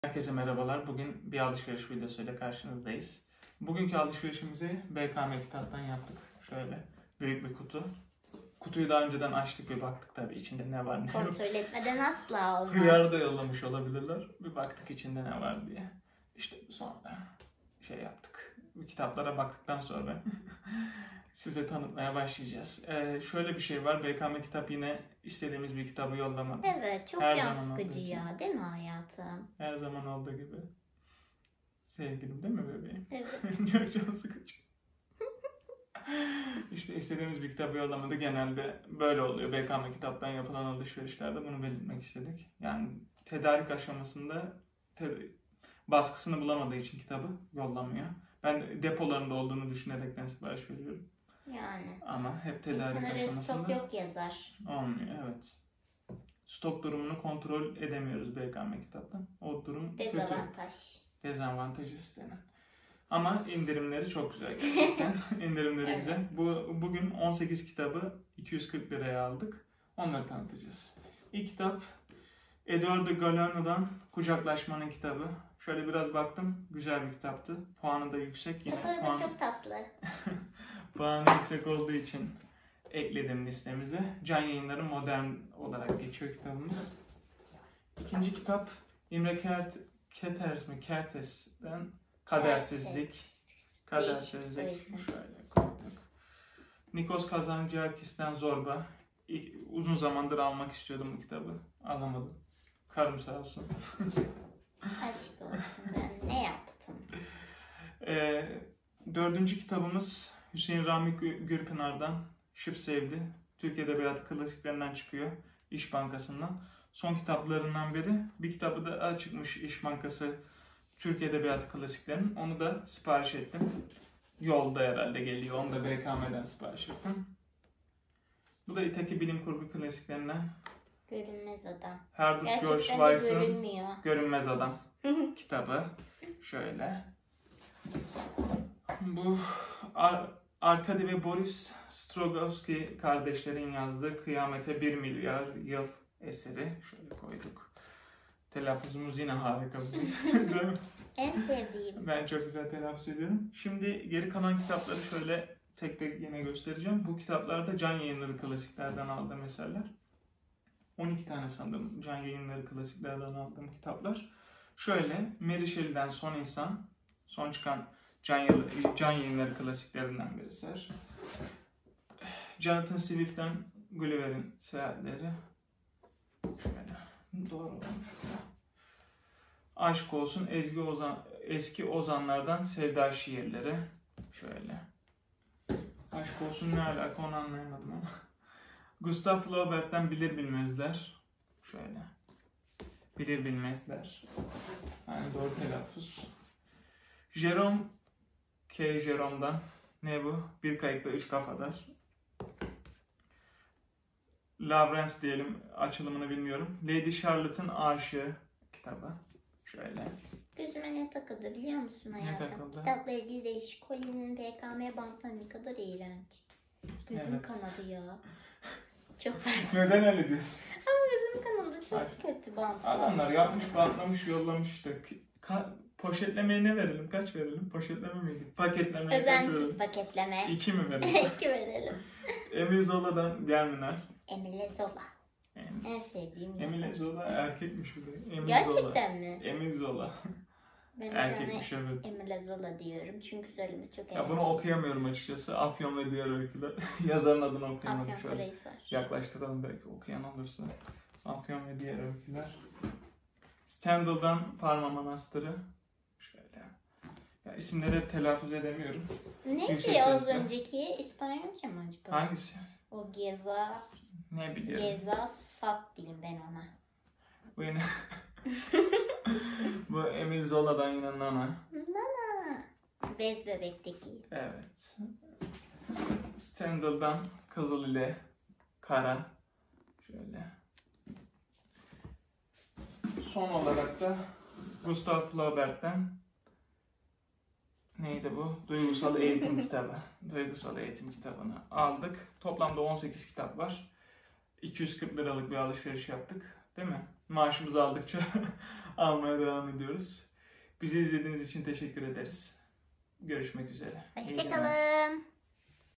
KİTAP ALIŞVERİŞİ 2-BKM KİTAP
Bkm kitaptan 18 kitap aldık ve 240 tl ödedik
Genel olarak Can Yayınları Klasiklerinden alışveriş yaptık.İyi seyirler